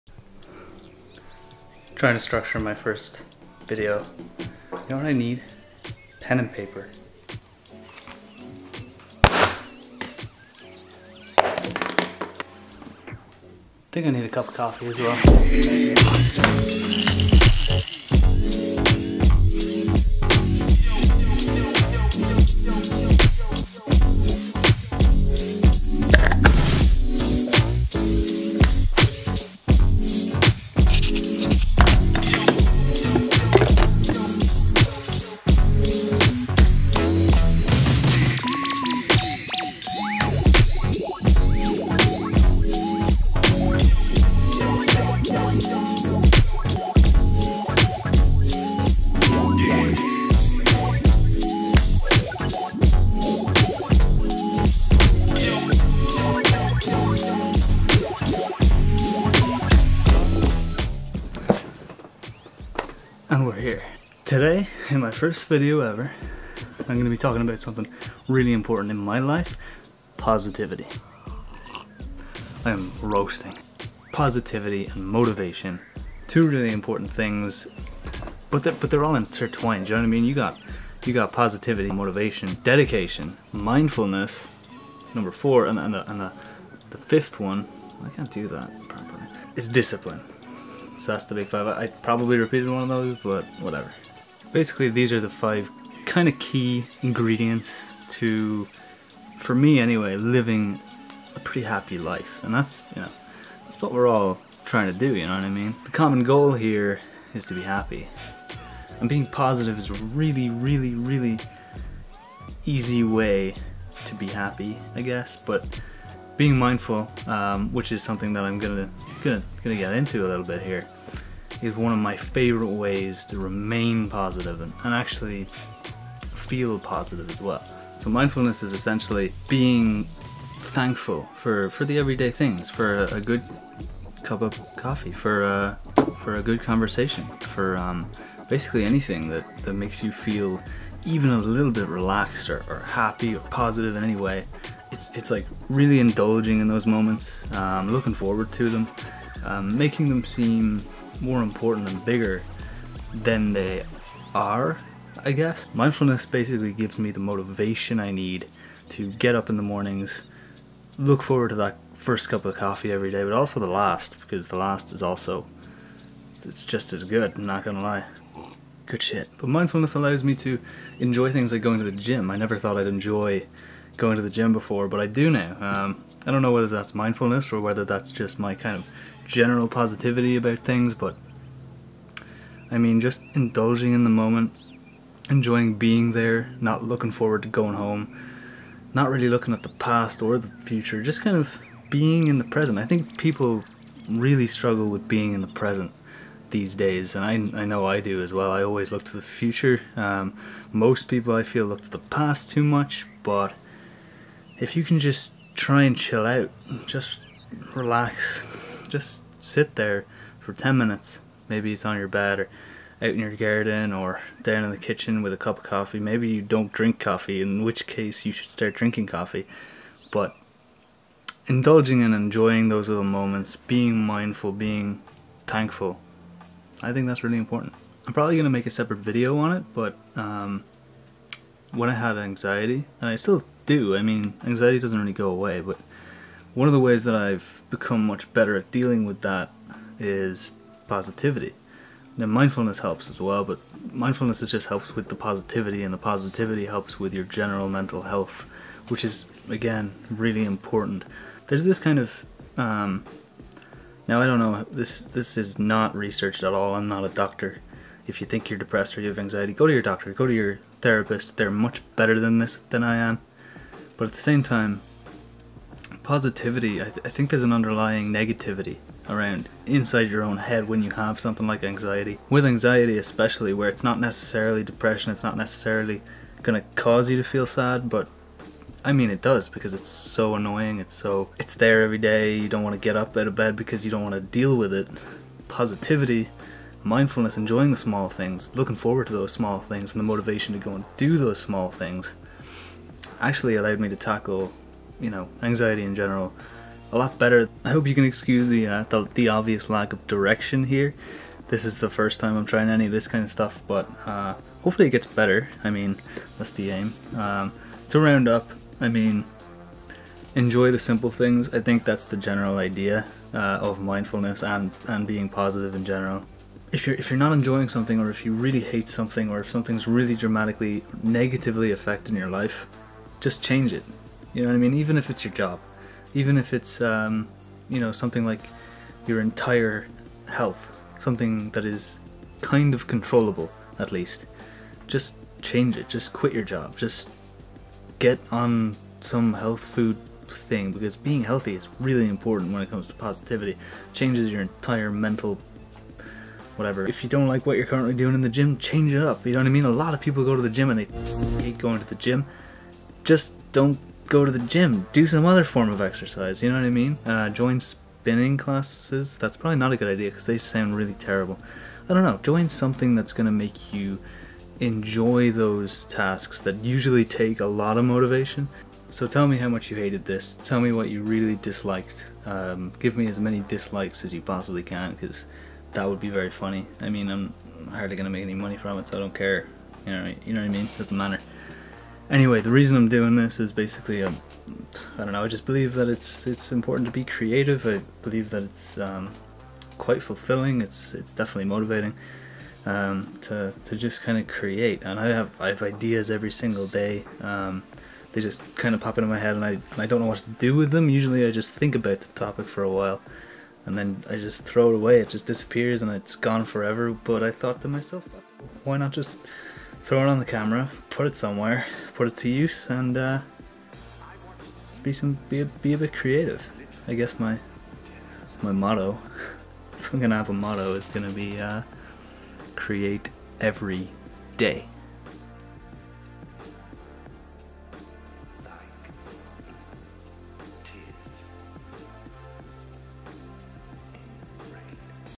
Positivity
Create Every Day.

It's so important to be positive these days, stop living in the past, and enjoy the simple things. This way of living can help you in more was than 1.

Camera used: Canon EOS M50 + Canon EF-M 15-45mm f/3.5-6.3 IS STM Lens + Rode VideoMicro.